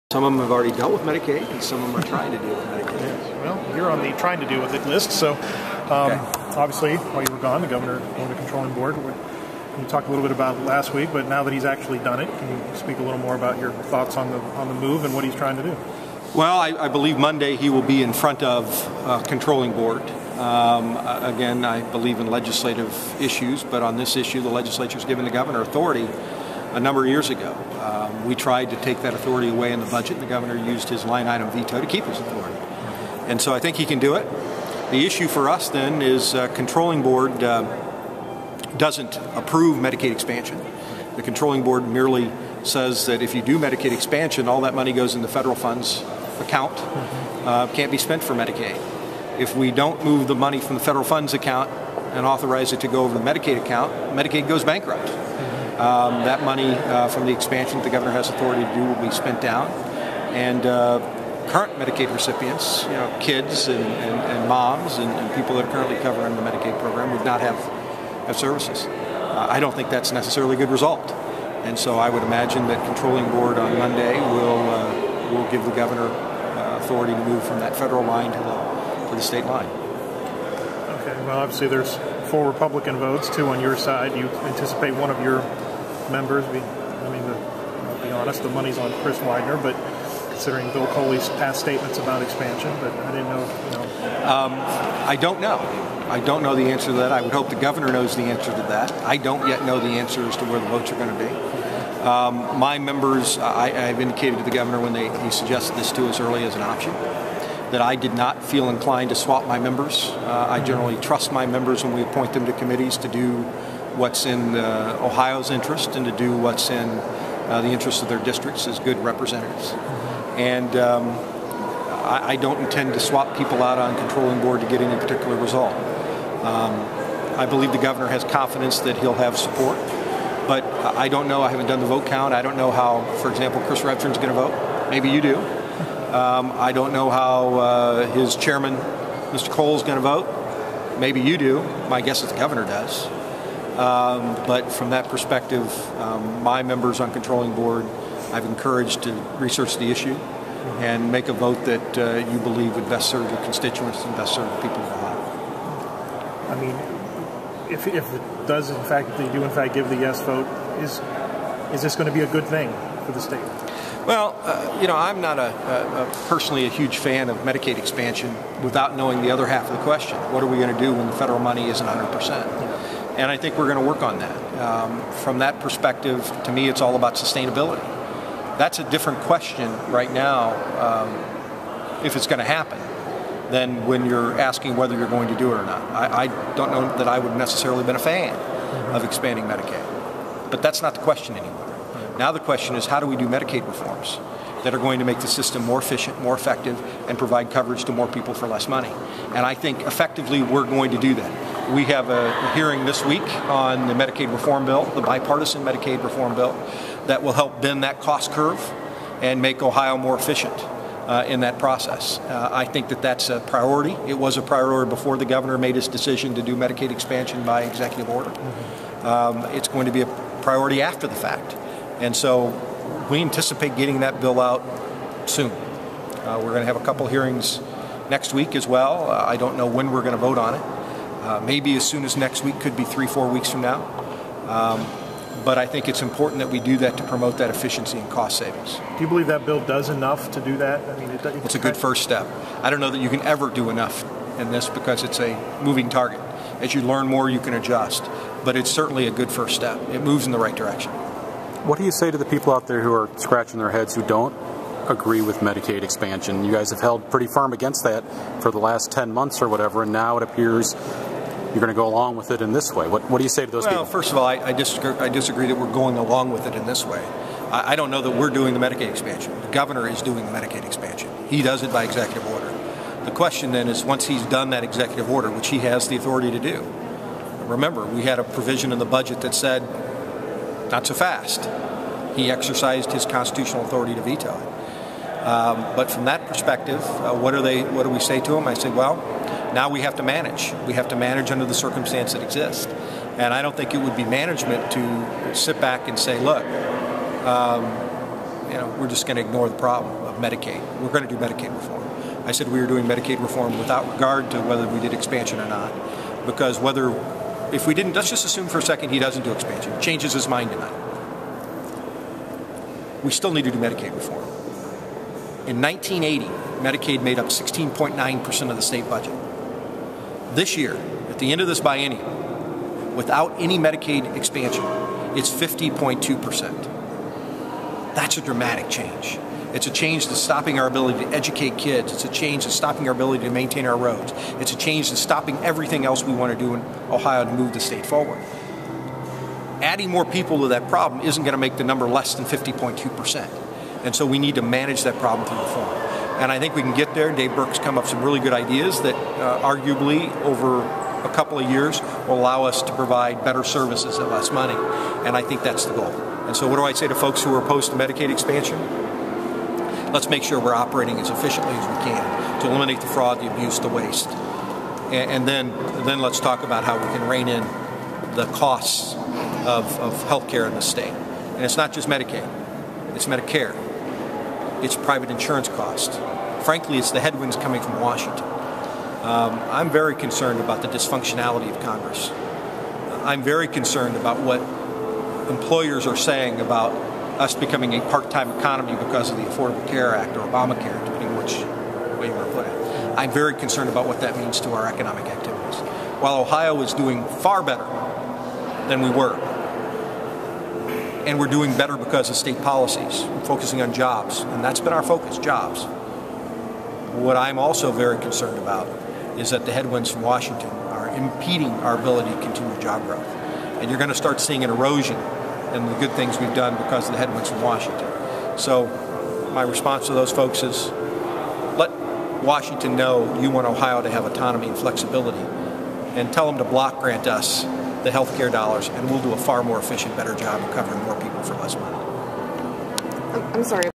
Faber on Medicaid Expansion 1 of 2
Republican Senate President Keith Faber answers reporters' questions about Gov. John Kasich's decision to use the state Controlling Board to expand Medicaid.